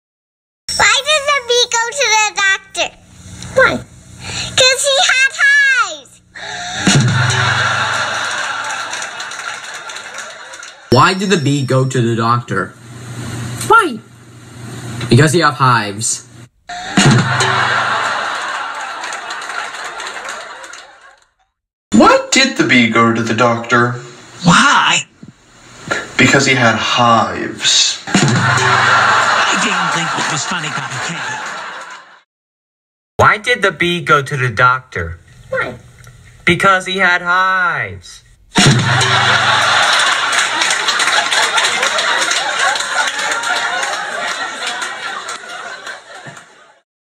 Baby Dolittle Bee Joke Compilation
These are the bee joke videos. This joke is told by Sierra, Antonio, Krazy Krok and Pavlov!